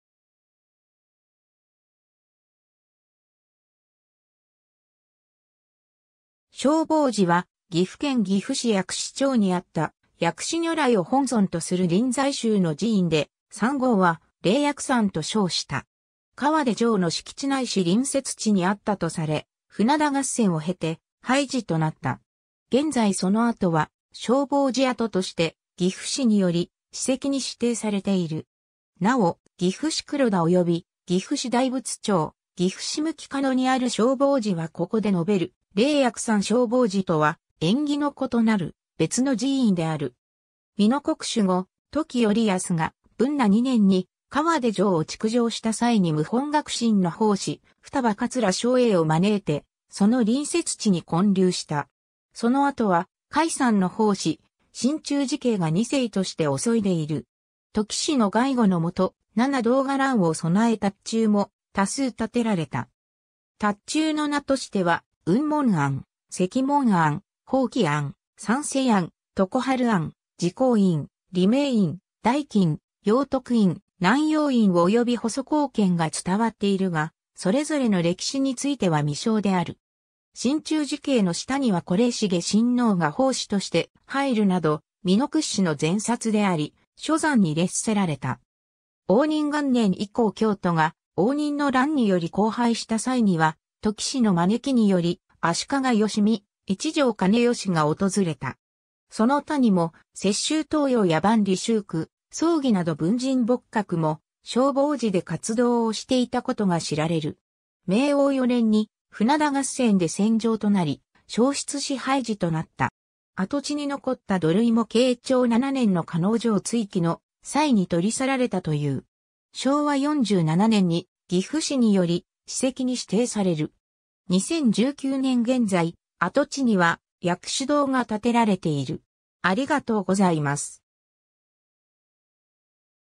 正法寺 (岐阜市薬師町)
正法寺（しょうぼうじ）は岐阜県岐阜市薬師町にあった薬師如来を本尊とする臨済宗の寺院で、山号は霊薬山と称した。
川手城の敷地ないし隣接地にあったとされ、船田合戦を経て廃寺となった。
現在その跡は正法寺跡として岐阜市により史跡に指定されている。
なお、岐阜市黒田（正法寺）及び岐阜市大仏町（正法寺）、岐阜市向加野にある正法寺はここで述べる霊薬山正法寺とは縁起の異なる別の寺院である。
 美濃国守護、土岐頼康が文和2年（1353年）に川手城を築城した際に無本覚心（法灯禅師、法灯派の祖）の法嗣、嫩桂正栄を招いてその隣接地に建立した。
その後は開山の法嗣、信中自敬が2世として襲いでいる。
土岐氏の外護のもと、七堂伽藍を備え塔頭も多数建てられた。
塔頭の名としては雲門庵、石門庵、法喜庵、三世庵、常春庵、慈光院、利名院、大亀院、陽徳院、南陽院及び細香軒が伝わっているが、それぞれの歴史については未詳である。
信中自敬の下には惟成親王が法嗣として入るなど美濃屈指の禅刹であり、諸山に列せられた。
 応仁元年（1467年）以降京都が応仁の乱により荒廃した際には土岐氏の招きにより足利義視、一条兼良が訪れた。
その他にも雪舟等楊や万里集九、宗祇など文人墨客も正法寺で活動をしていたことが知られる。
 明応4年（1495年）に船田合戦で戦場となり、焼失し廃寺となった。
跡地に残った土塁も慶長7年（1602年）の加納城築城の際に取り去られたという。
昭和47年（1972年）に岐阜市により史跡に指定される。
2019年現在、跡地には薬師堂が建てられている。